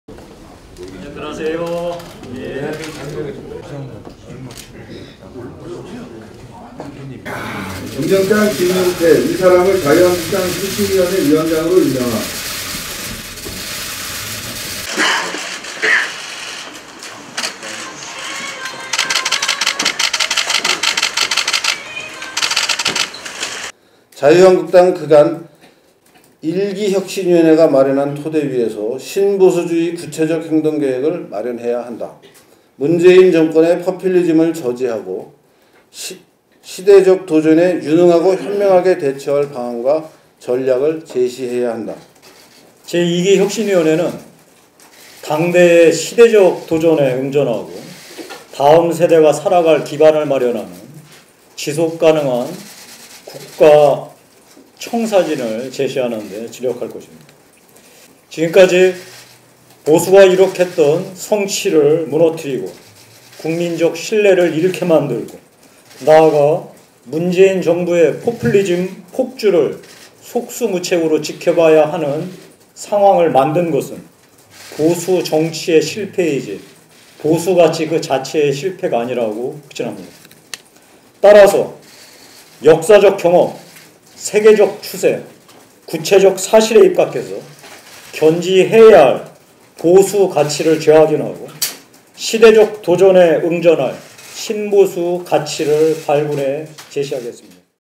홍준표 대표, 류석춘 이어 김용태 혁신위원장 임명
홍준표 자유한국당 대표가 28일 오전 서울 여의도 당사에서 열린 혁신위원장 및 지방선거기획단 임명장 수여식에 참석해 김용태 신임 혁신위원장에게 임명장을 수여한 뒤 기념촬영을 하고 있다.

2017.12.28 일요서울TV 장원용 기자